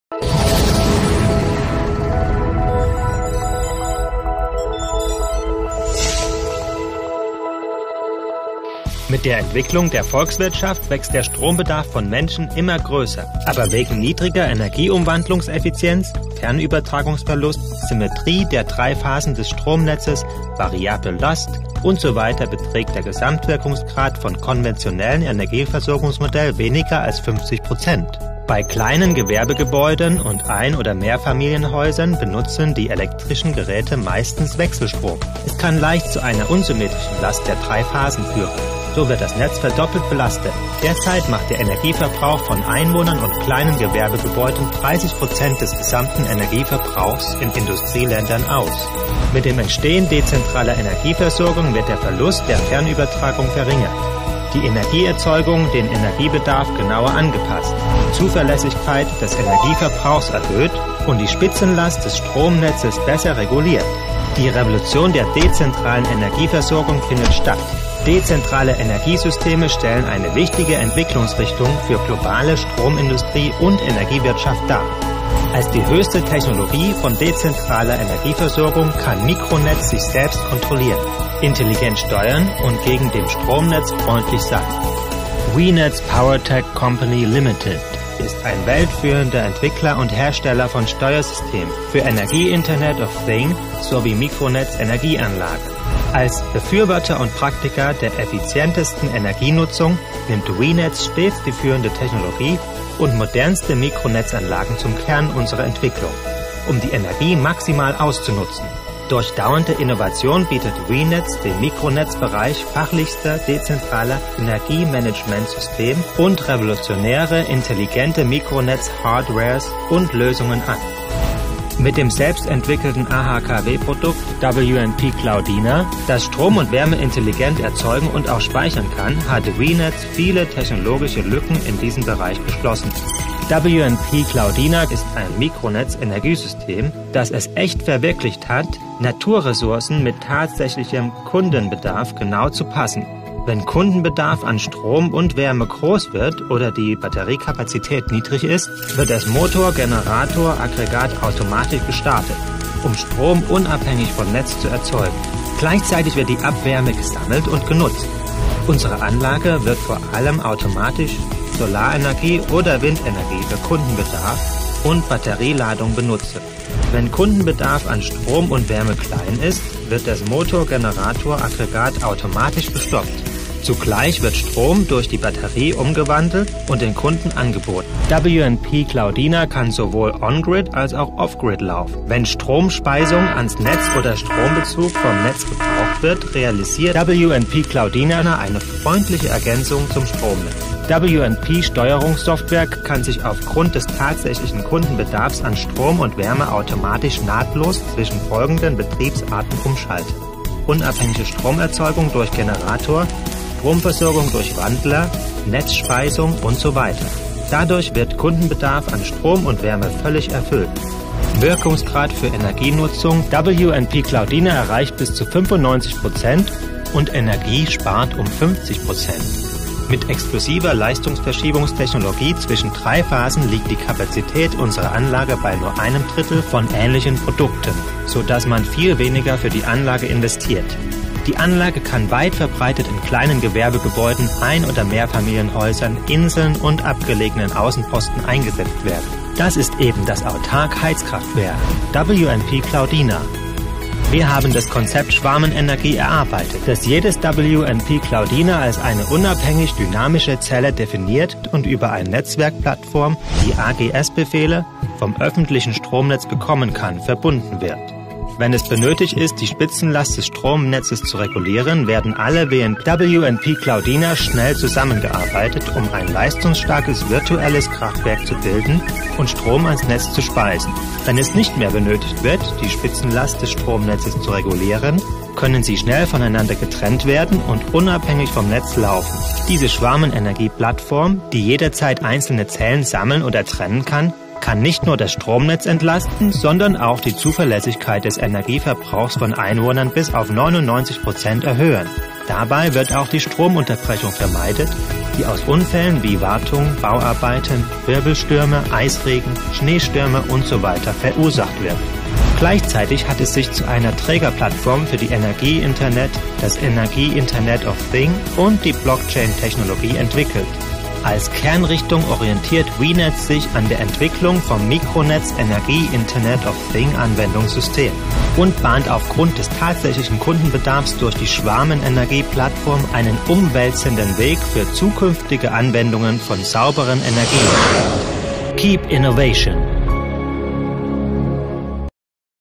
Autonomes Mini-Kraftwerk EN-Cloudia
Die Zukunft der privaten und autonomen Strom- und Wärmekrafterzeugung "invented in China & Germany"